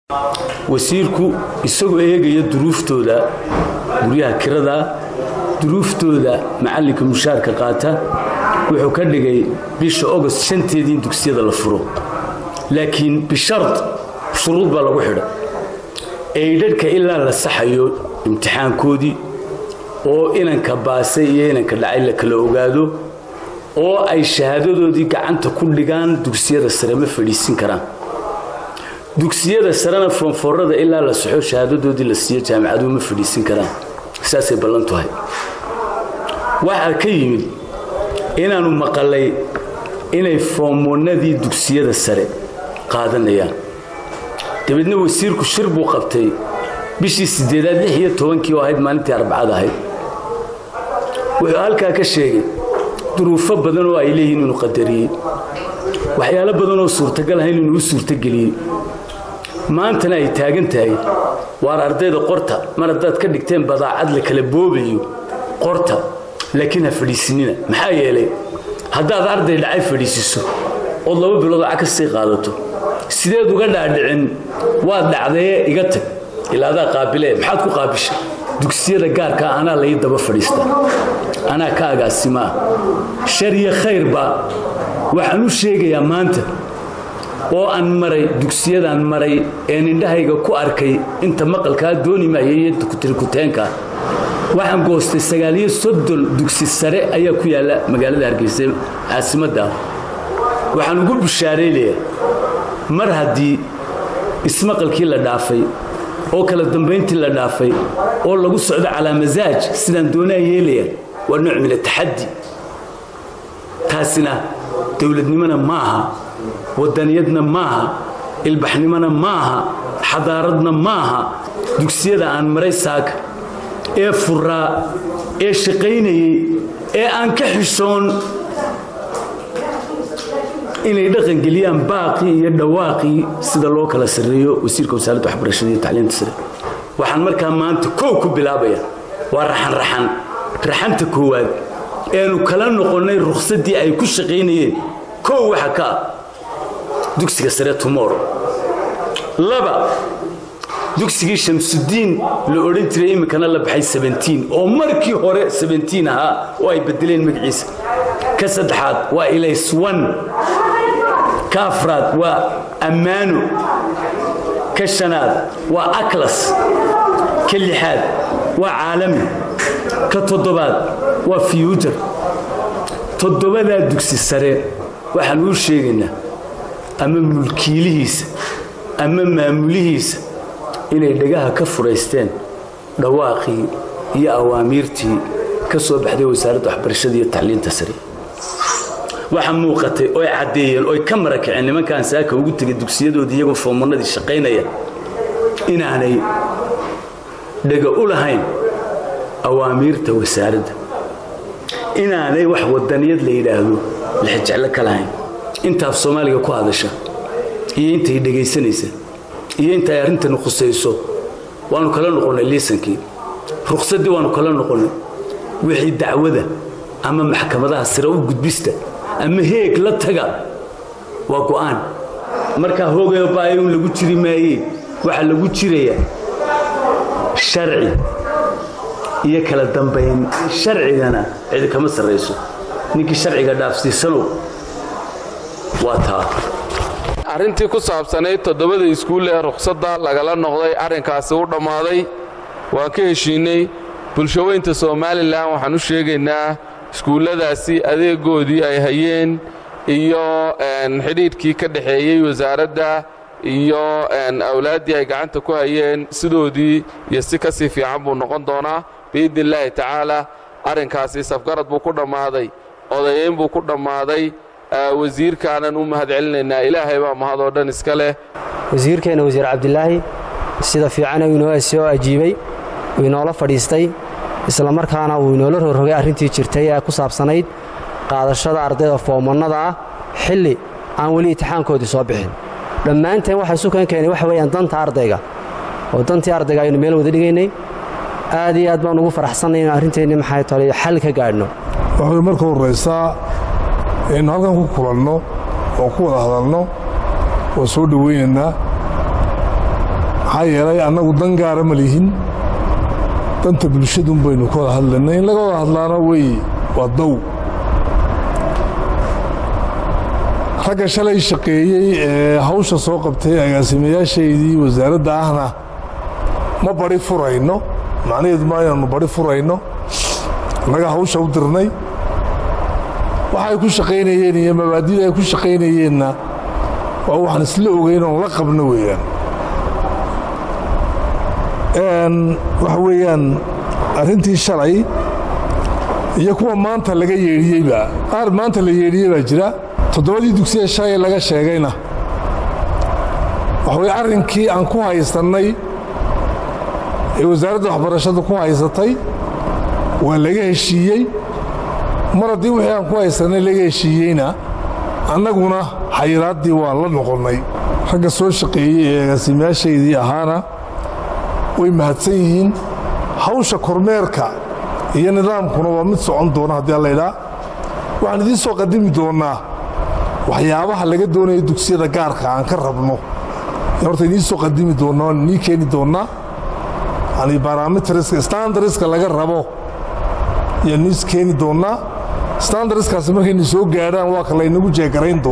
Wasaarada Waxbarashada Somaliland Oo Ka Noqotay Go'aankii Ay Ka Soo Saartay 7-da School
Waxaanu Ruqsadii kala Noqonay 7 School Iyo Heshiis Cusub W/ Waxbarashada Iyo Dugsiyada Gaarka ah